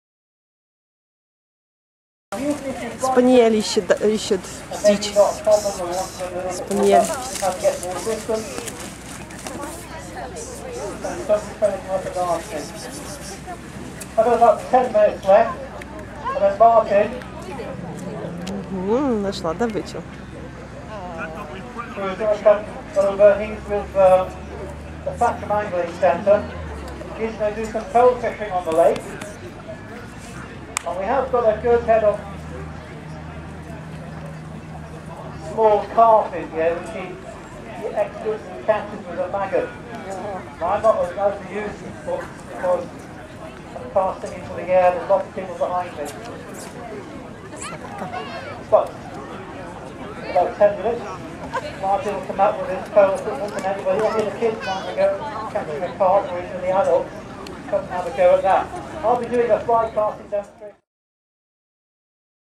Spaniel
Short spaniel work demo on the Royal Berkshire County Show